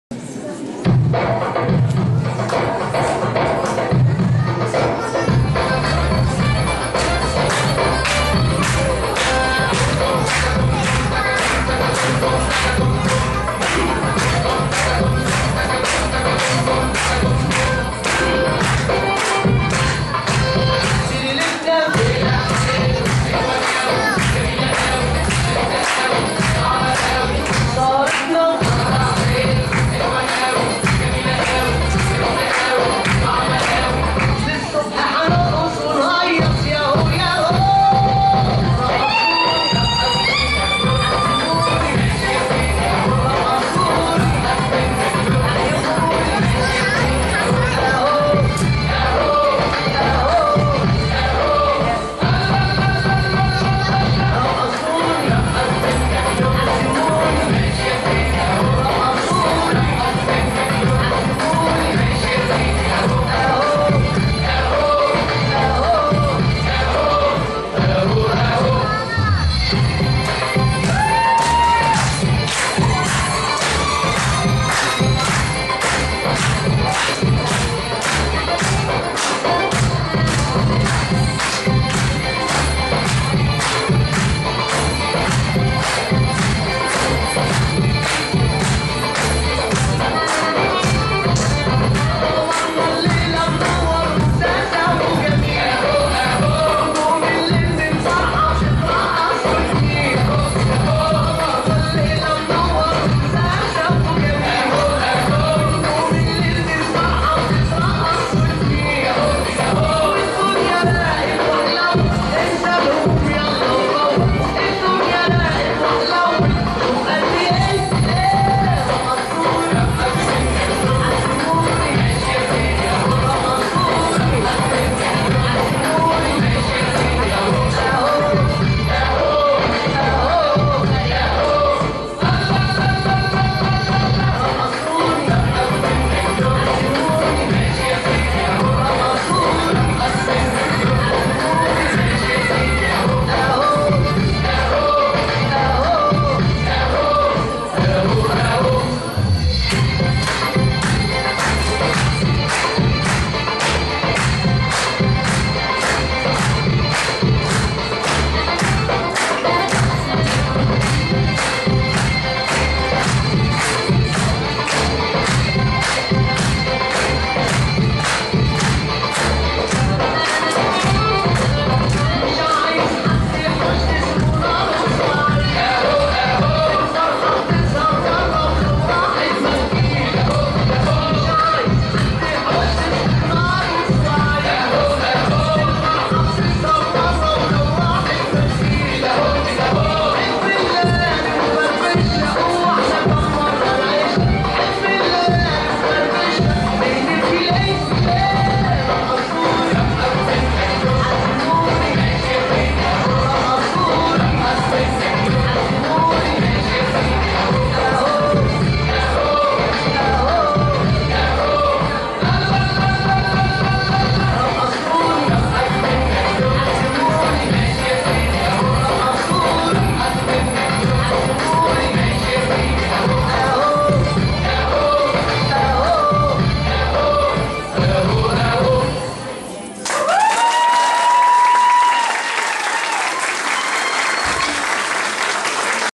Chá Flor de Lótus 26/10/14 - Saidi Moderno
Modalidade: Dança do Ventre
Professora: Bella Pedroso
Alunas: Morgana, Rosangela, Tassiane e Ivone
Estúdio Flor de Lótus - Curitiba PR
Rua João Betega, 499 - Portão, Curitiba - PR, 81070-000